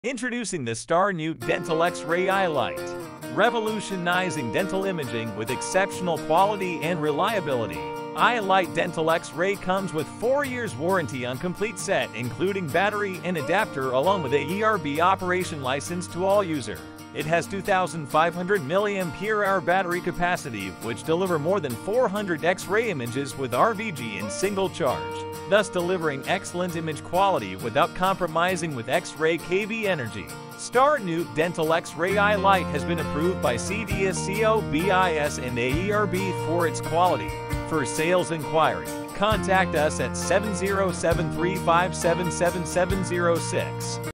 i Lite Dental X Ray best for Dental Imaging with 4 years warranty and lowest Price
Introducing the Star Nuke Dental X-Ray i-Lite: Revolutionizing dental imaging with exceptional quality and reliability. i-Lite Dental X Ray comes with 4 years warranty on complete set including battery and adaptor along with A E R B Operation License to all user. It has 2500 milliampere hour battery capacity which deliver more than 400 X-Ray images with RVG in single charge. Thus delivering excellent image quality without compromising with X-Ray KV energy. Star Nuke Dental X-Ray i-Lite has been approved by C D S C O, B I S and A E R B for it's quality.